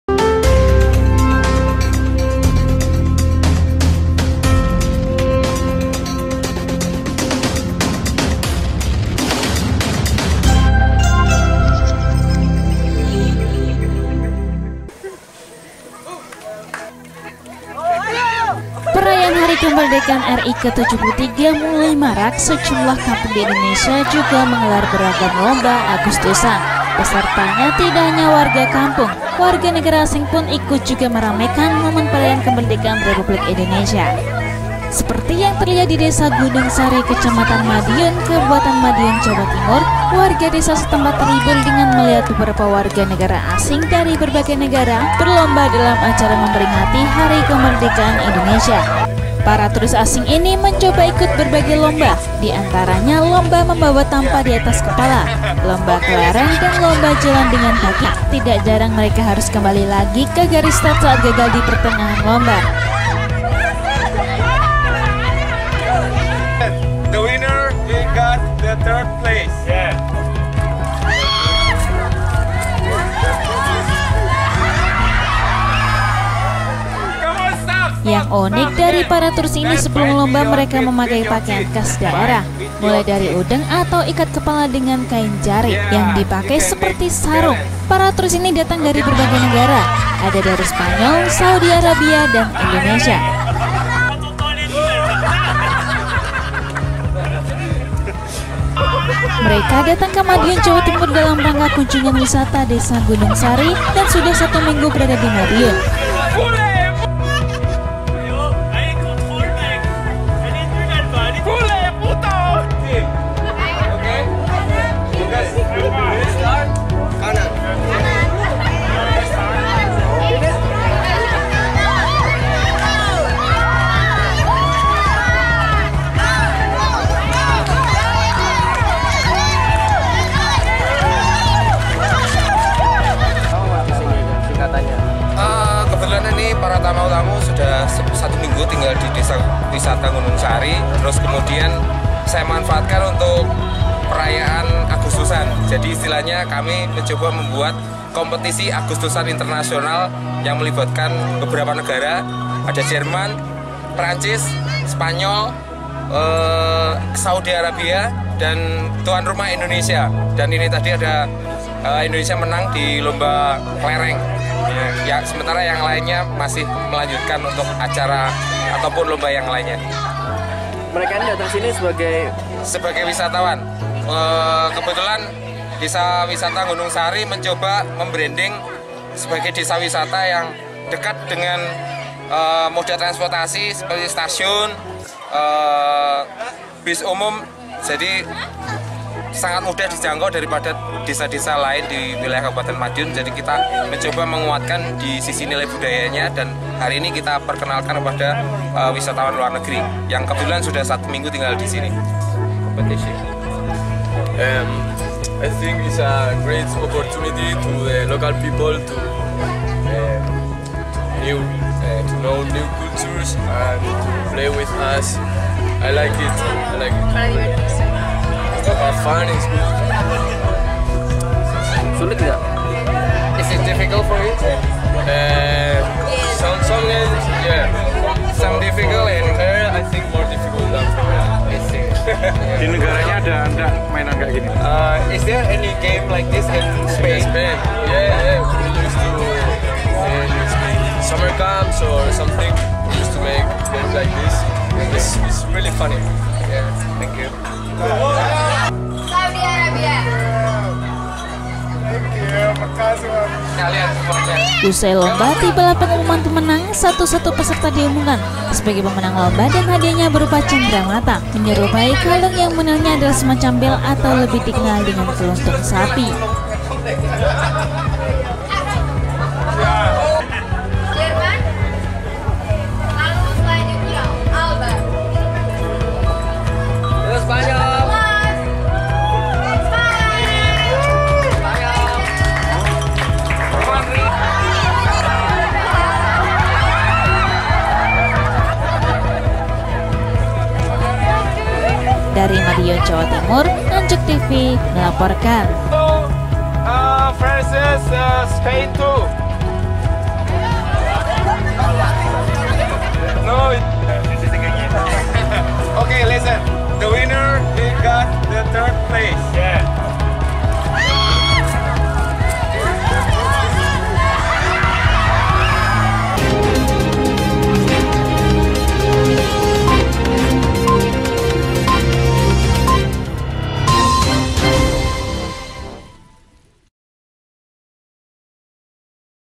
Lucu Poll, Bule Ikut Lomba Agustusan - Madiun | Anjukzone
SAUDARA, PERAYAAN HARI KEMERDEKAAN RI KE 73, MULAI MARAK, SEJUMLAH KAMPUNG DI INDONESIA, JUGA MENGGELAR BERAGAM LOMBA AGUSTUSAN, PESERTANYA TIDAK HANYA WARGA KAMPUNG, WARGA NEGARA ASING PUNG IKUT JUGA MERAMAIKAN, MOMEN PERAYAAN KEMERDEKAAN REPUBLIK INDONESIA.